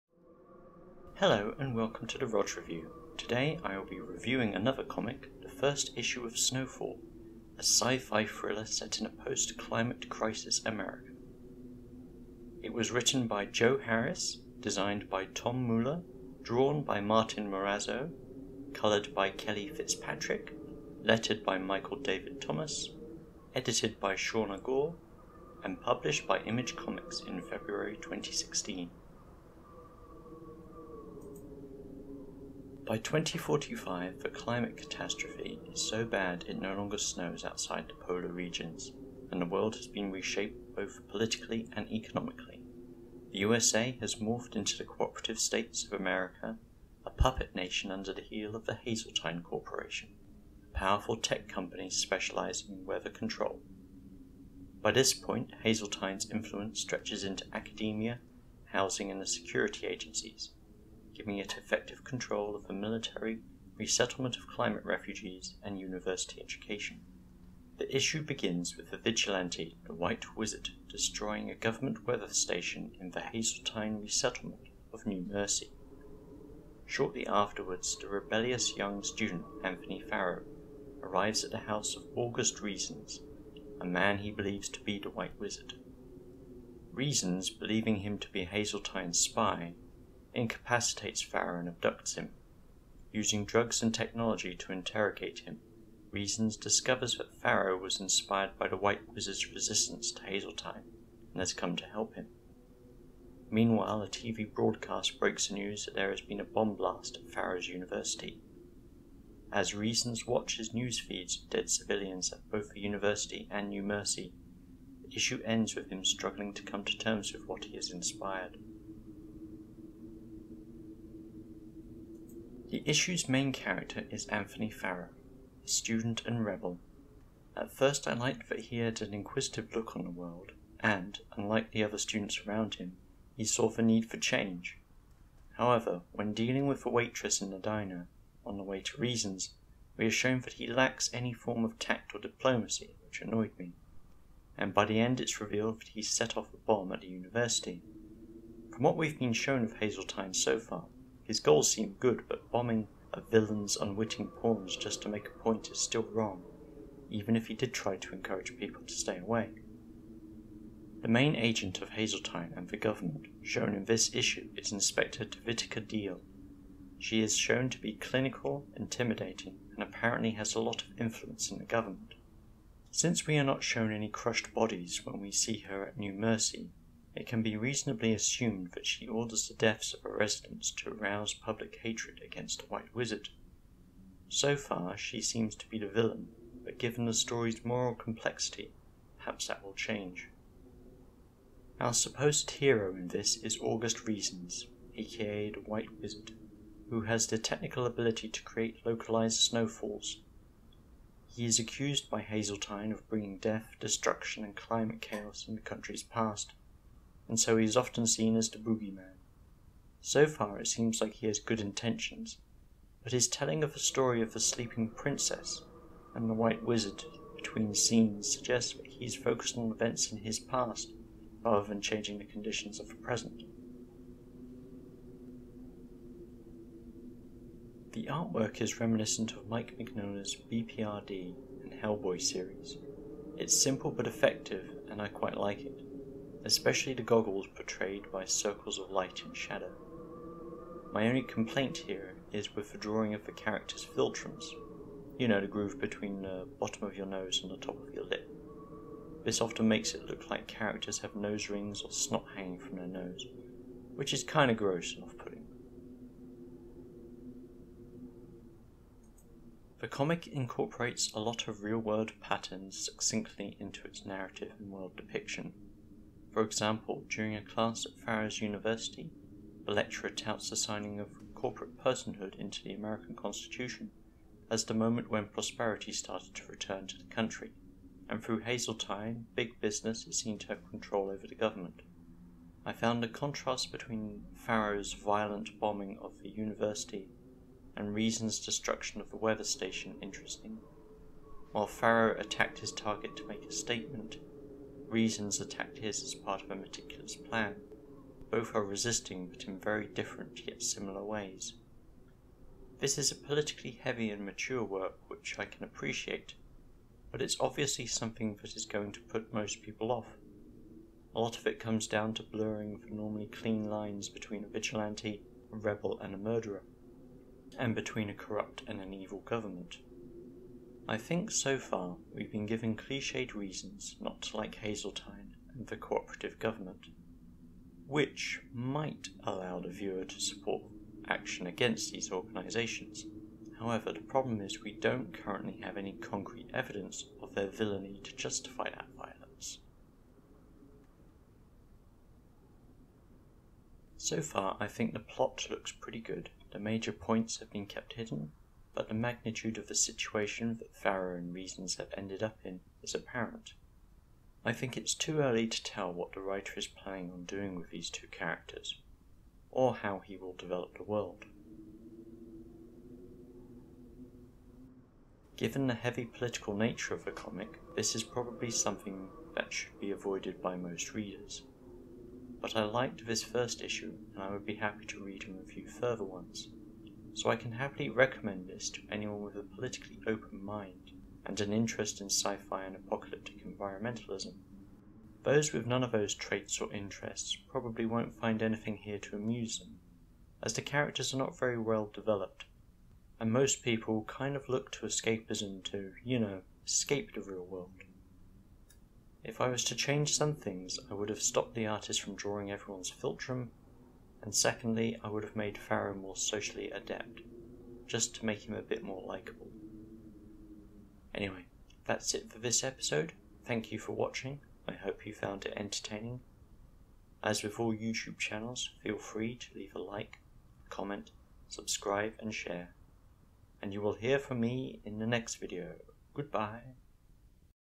Snowfall: Issue 1 Weather is a Weapon - Comic Review
A review of the 1st issue of the Image comic Snowfall.

 - All other images taken from Snowfall: Issue 1 for the purpose of review